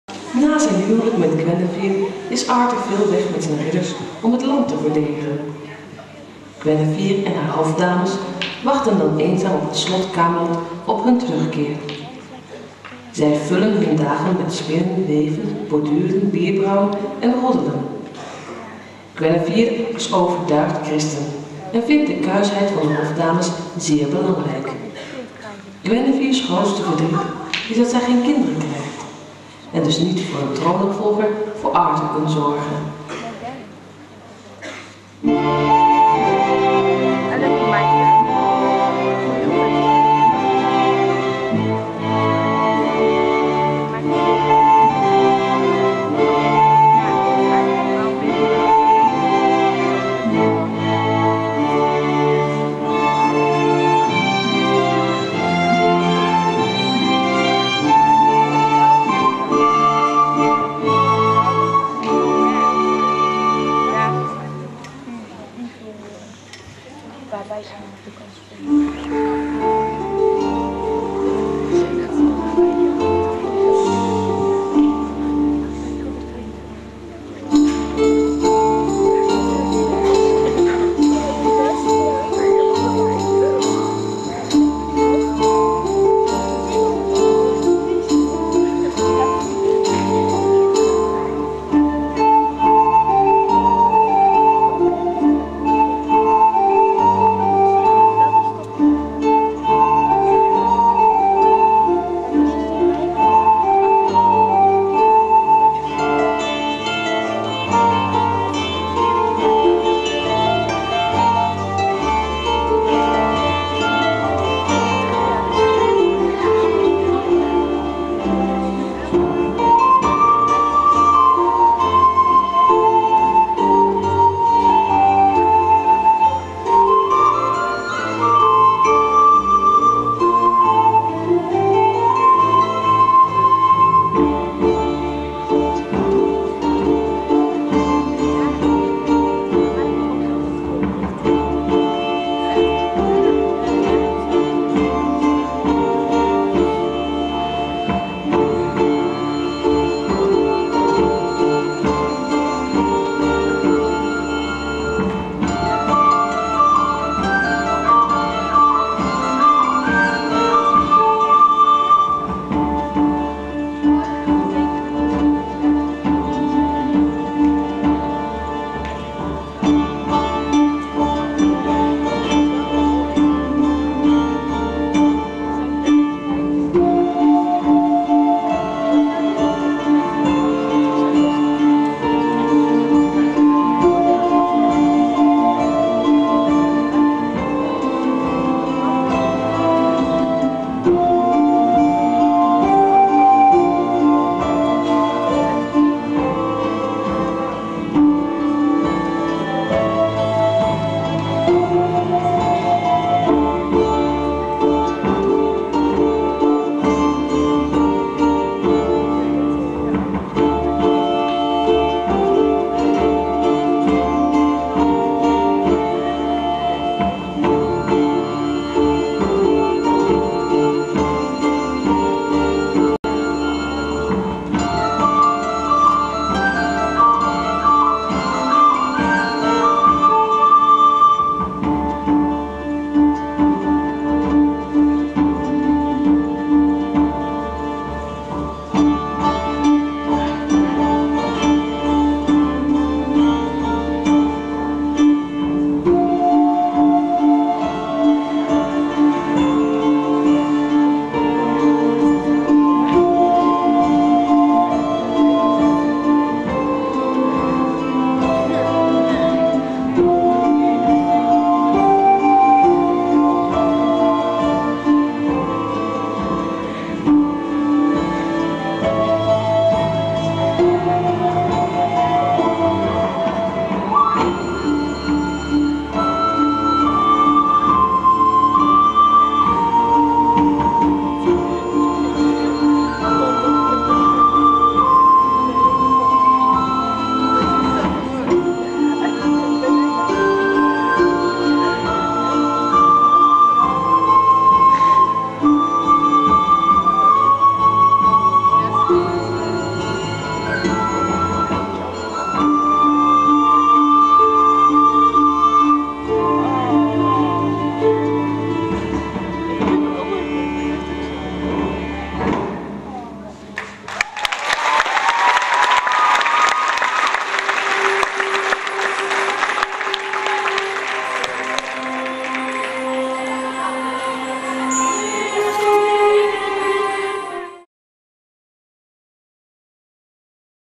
Kellie als Guinevere in King Arthur 2003
Scene uit de schoolmusical King Arthur gespeeld door leerlingen van het Newman College op 13 maart 2003